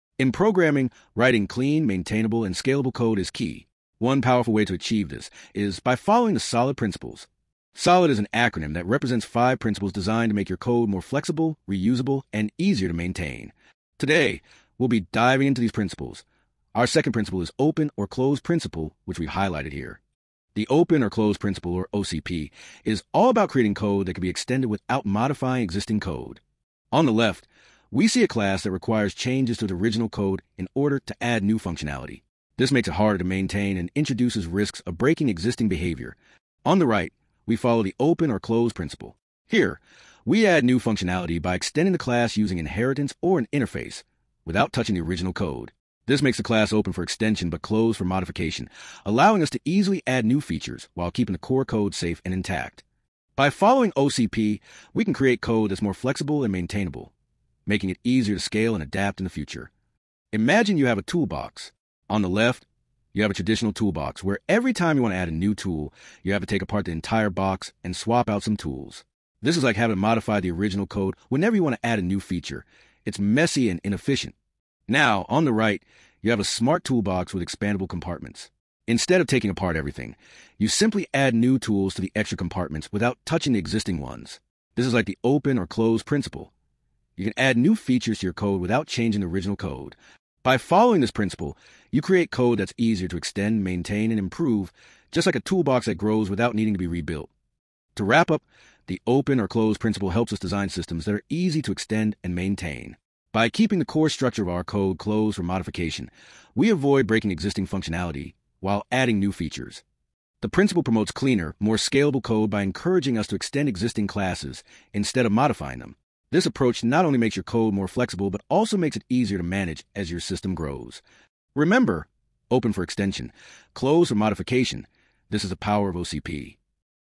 Master the Open/Closed Principle: Build Scalable Code! 💡
Want to write scalable and maintainable code? 🌟 Learn the Open/Closed Principle (OCP) and discover how you can add new features without changing existing code! By following OCP, you create code that’s open for extension but closed for modification, making your software easier to grow and maintain. Let’s dive into OCP and transform your development approach!